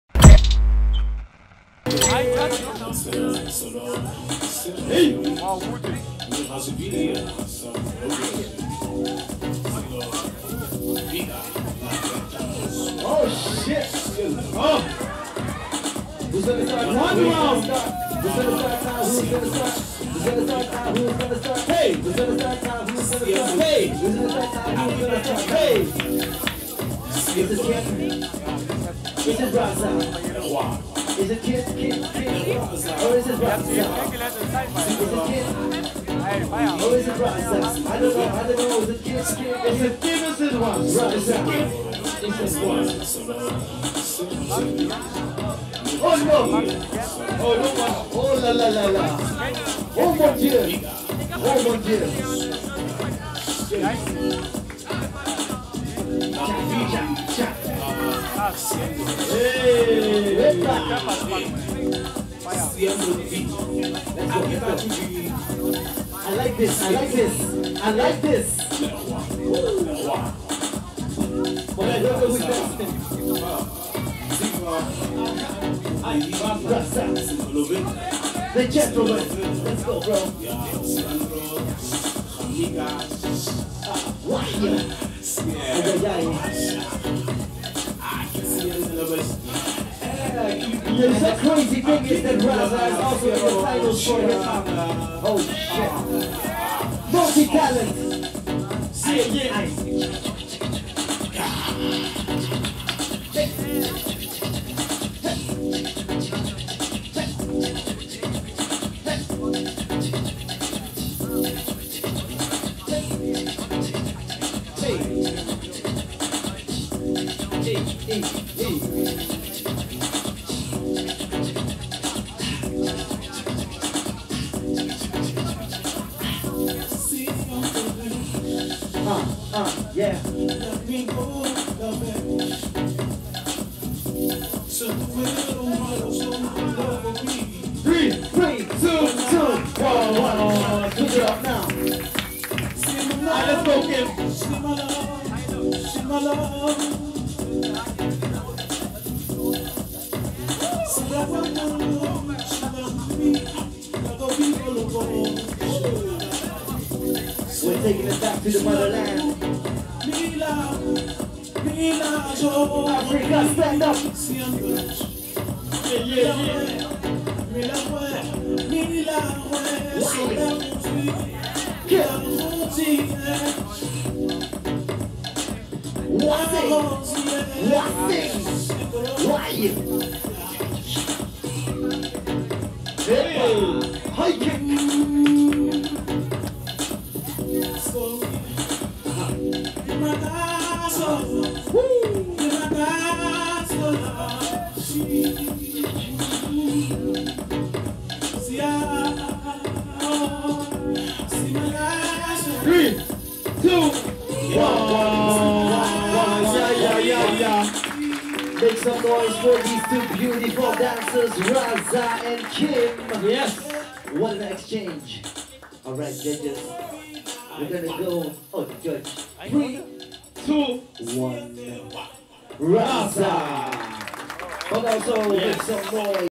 KIM VS RAZA | TOP16 HOUSE | THE KULTURE OF HYPE&HOPE | WATER EDITION S3
TOP16 HOUSE | 5-1-2019 | WATER edition S3 (2/4)
The Kulture of Hype&Hope 2019

Shout out to our team & partners:
DOX - FCP - Founder/Main organizer: Giovani Vreede @GIOVANIDIMENSION - Co Founder/Main organizer: Marsha Webster - organizer: Stash Ferdinand - Production Leader: Marieke van der Waal & All of Team Hype & DOX for making this event possible.

DJ: CRASH
JUDGES: Kwame Ba
HOSTS: Rajiv & Darren